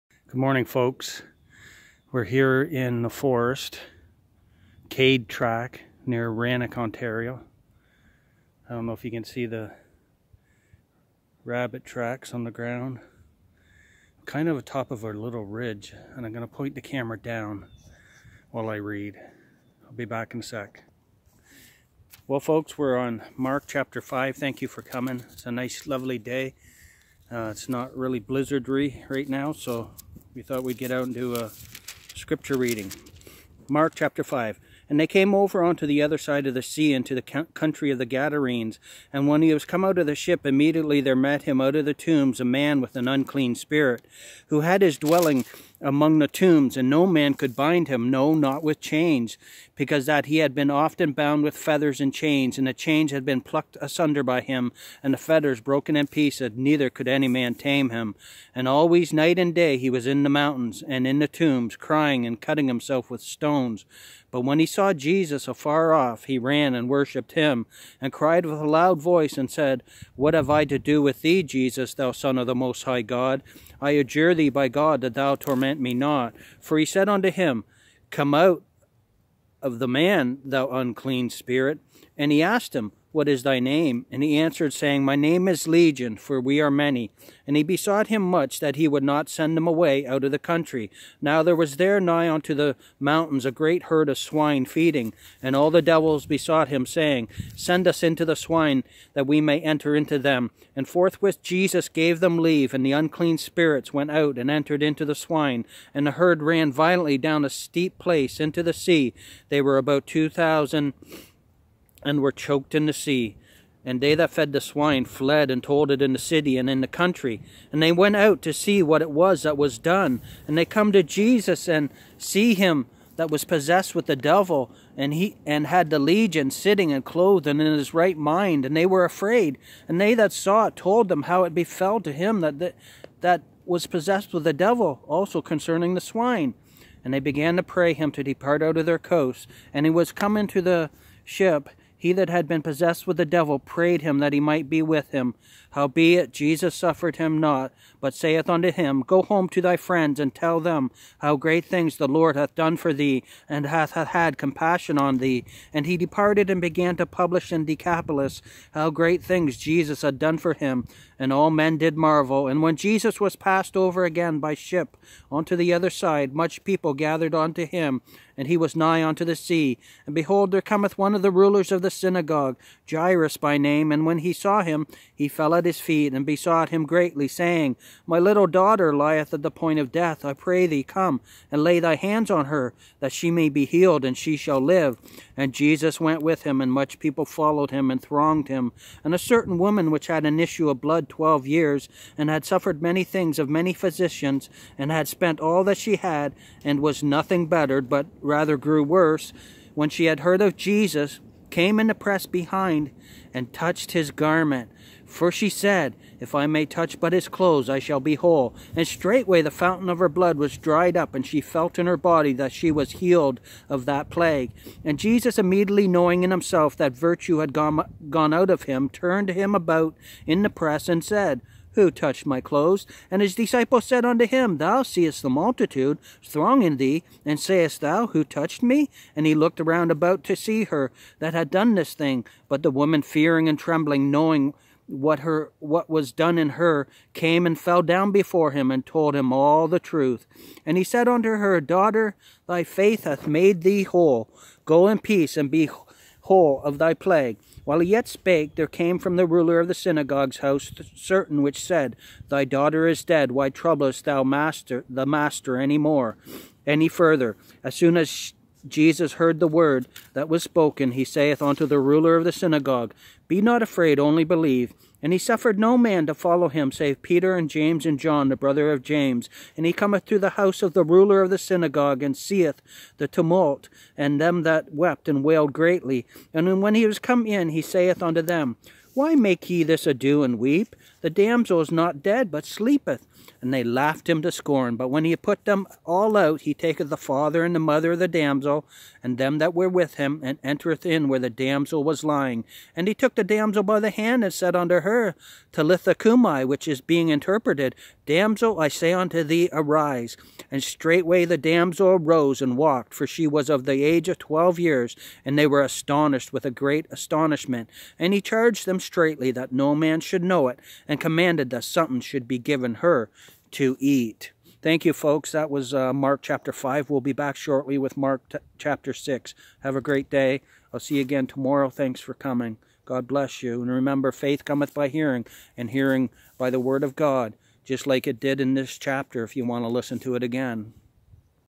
MARK 5 "BUT WHEN HE SAW JESUS AFAR OFF, HE RAN AND WORSHIPPED HIM."
mark5 bible newtestament gospel worshipped afar SCRIPTURE READING FROM MARK CHAPTER FIVE OF THE NEW TESTAMENT OF THE HOLY BIBLE.